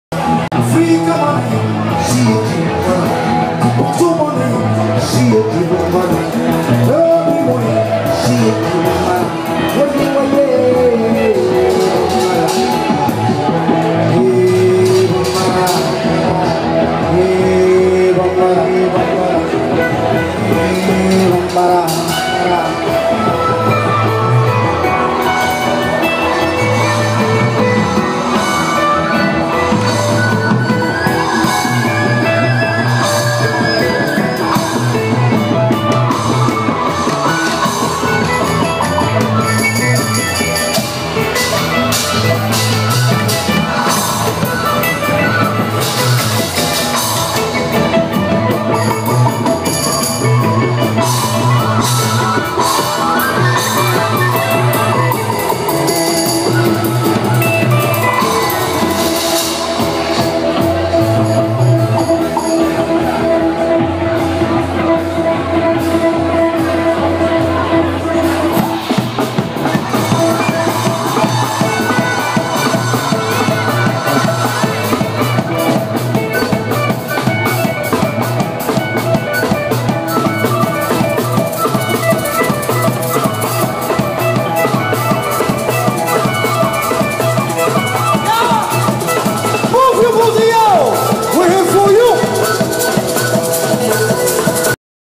Gnawa Blues All-Stars live @ The Old Market Assembly, Bristol
Gnawa Blues All-Stars live at the Old Market Assembly, Bristol on 13th February 2016 as part of BBC 6 Music fringe festival. Simo Lagnawi with Rafa from Brazil and Laurence on guitars, Chris on drums & special guest Ricardo de Noronha on bamboo flute.